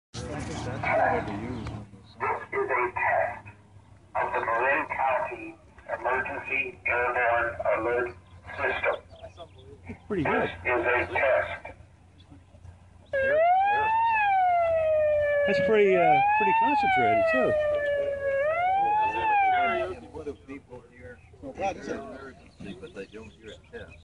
Marin County Sheriff's T-41 with Loud Hailer system
recorded while a/c 1/2 mile away, 1,000 ft